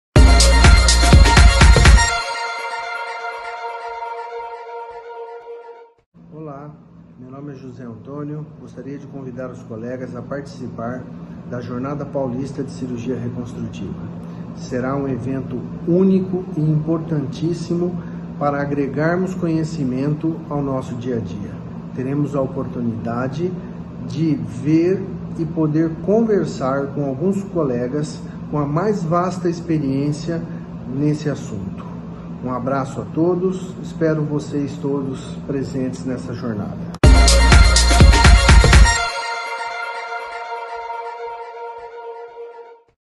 JPr 2020 - Dr José Antonio Sanches convida
Dr. José Antonio Sanches, de Catanduva (SP), convida para a Jornada Paulista Reconstrutiva – JPr 2020. O cirurgião plástico participa de Mesa Redonda sobre Reajuste do Corpo Superior, como moderador.

O evento acontece entre 11 de setembro e 02 de outubro, em plataforma virtual. Os conteúdos são lançados toda terça-feira e sexta-feira, a partir das 17h, e ficam disponíveis por tempo limitado.

Veja a programação completa no site da Regional São Paulo. Inscrições abertas.